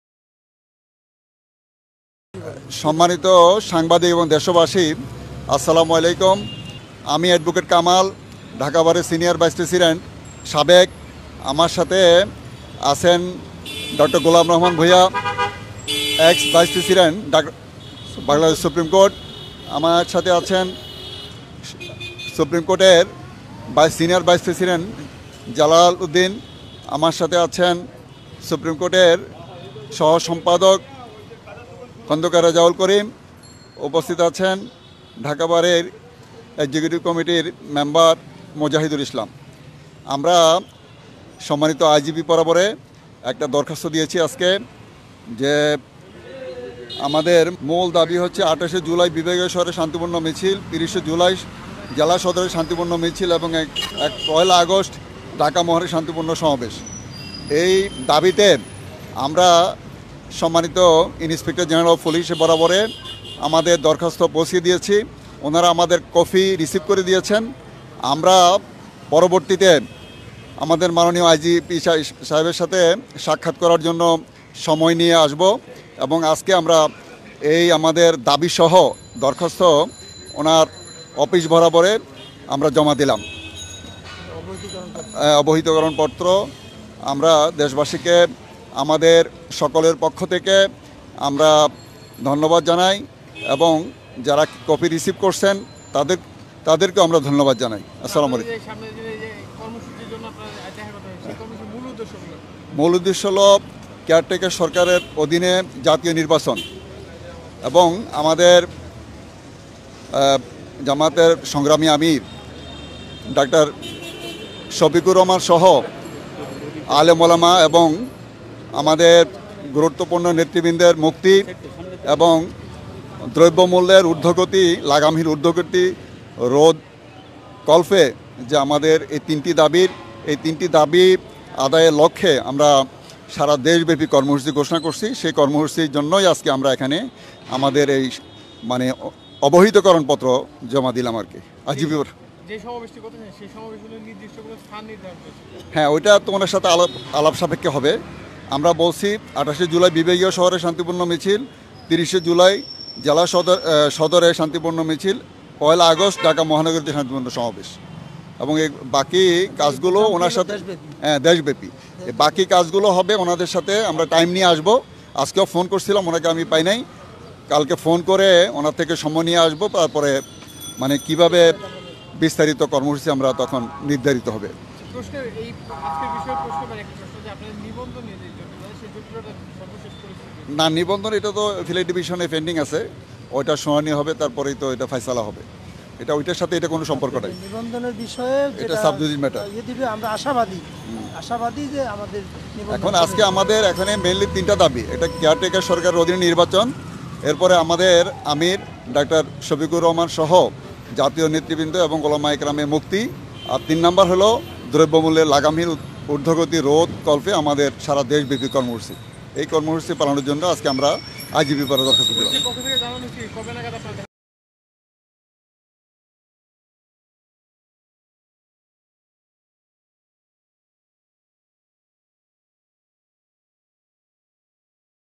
কর্মসূচী বাস্তবায়নের জন্য আইজিপি কার্যালয়ে জামায়াতের প্রতিনিধি দল
বাংলাদেশ জামায়াতে ইসলামীর কেন্দ্র ঘোষিত কর্মসূচী আগামী ২৮ ও ৩০ জুলাই দেশব্যাপী বিক্ষোভ মিছিল এবং ১লা আগষ্ট ঢাকায় সমাবেশে সহযোগিতার আবেদন নিয়ে আজ ২৪ জুলাই সোমবার বিকেল ৫-০০টায় বাংলাদেশ পুলিশের আইজিপি কার্যালয়ে যান বাংলাদেশ জামায়াতে ইসলামীর একটি প্রতিনিধি দল।